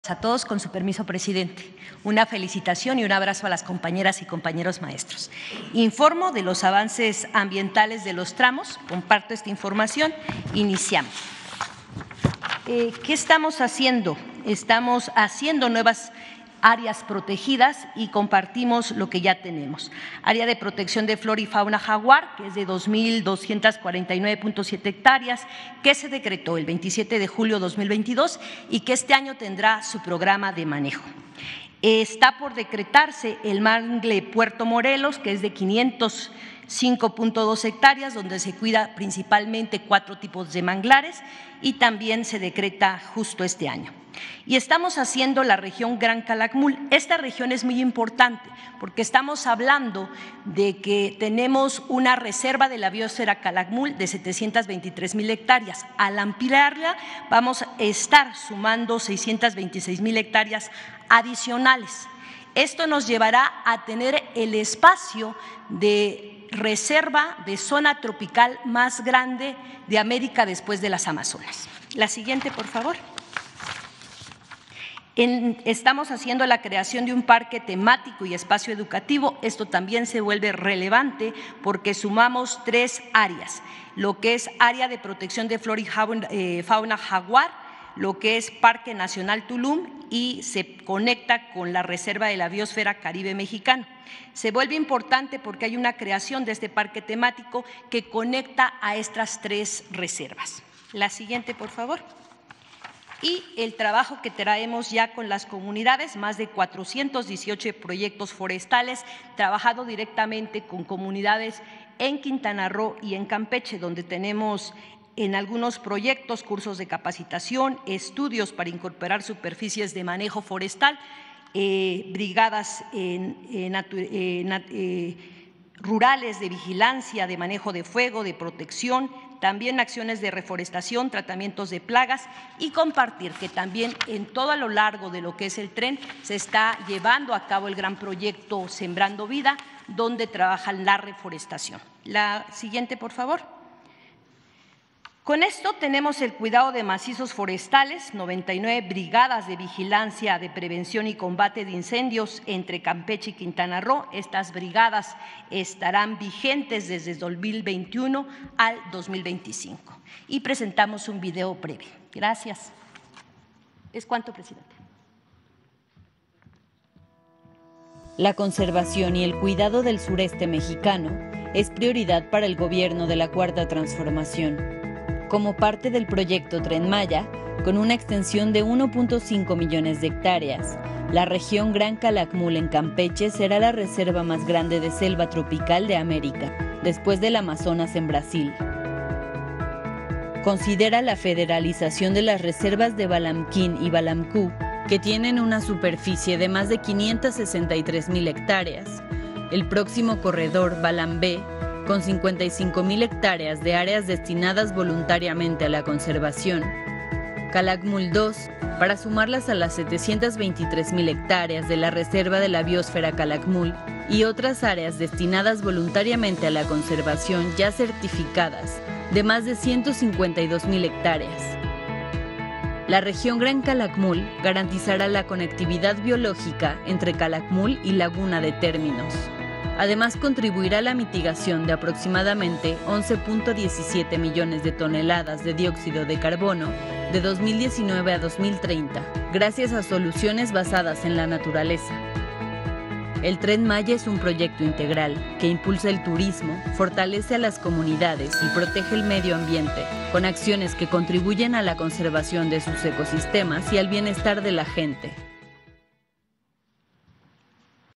Tren Maya#Tramos 5,6,7#Protección Ambiental#María Luisa Albores 15 mayo 2023
MARÍA LUISA ALBORES GONZÁLEZ, SECRETARIA DE MEDIO AMBIENTE Y RECURSOS NATURALES